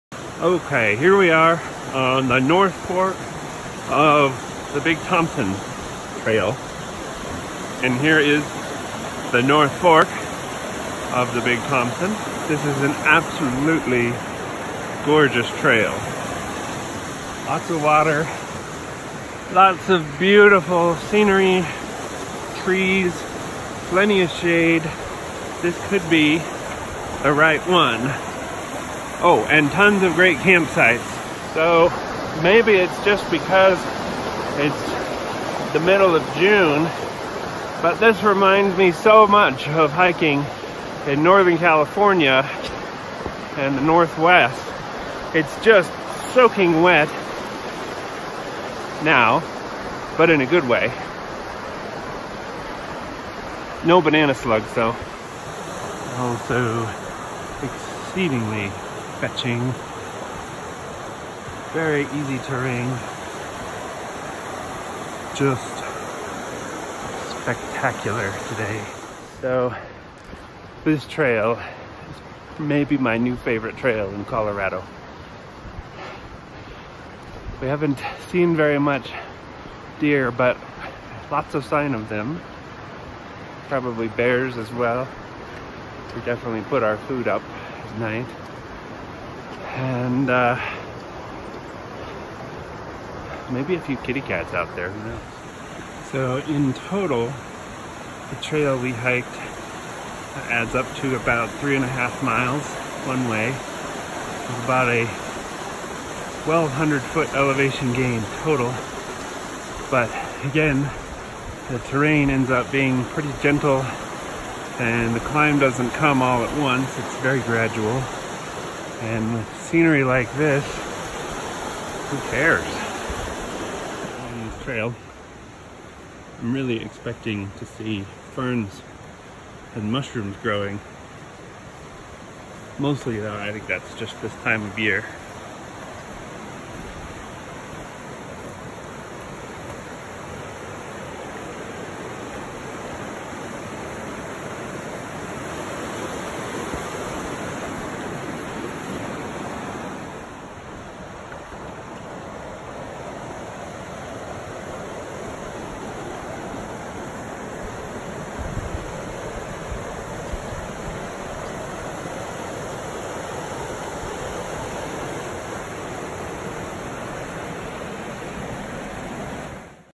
North Fork Big Thompson Trail 2019-06-23
Brief walking description of the Big Thompson North Fork Trail